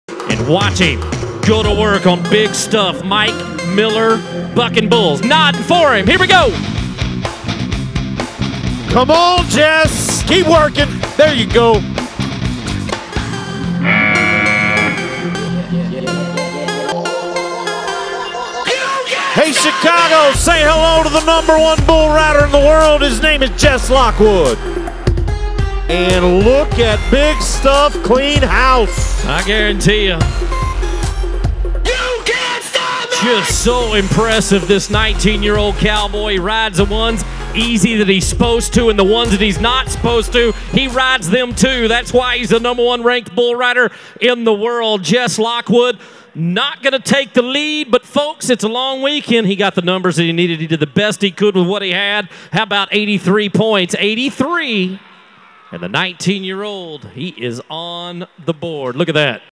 Jess Lockwood rides Big Stuff for 83 points
Jess Lockwood rides Big Stuff for 83 points in Round 1 of the 2017 PBR Built Ford Tough Series in Chicago, IL